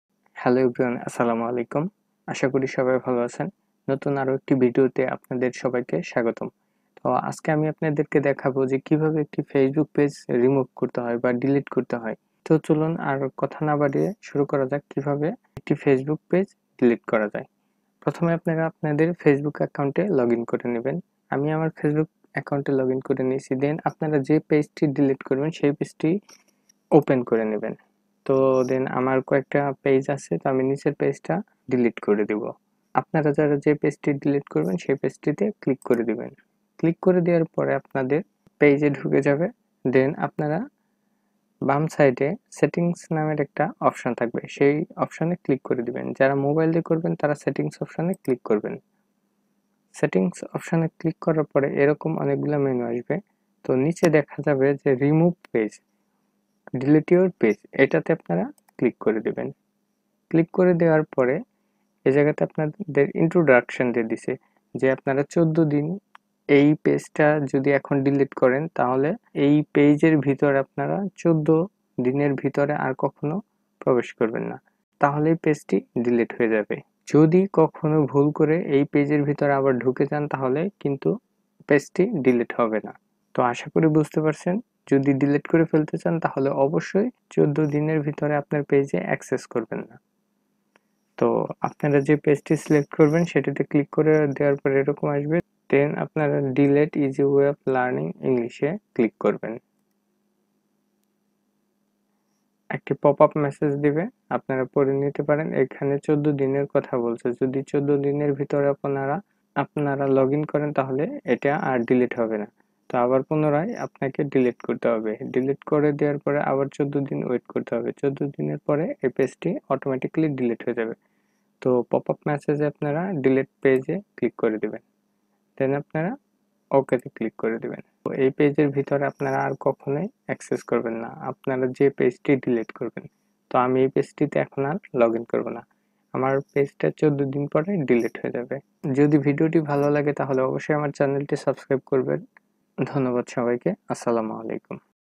How to Delete Facebook page
In this video, I will show how to delete a Facebook page very easily. please watch this full video without a skip. so let's start.

How to delete Facebook page | delete Facebook page | delete FB page Easily.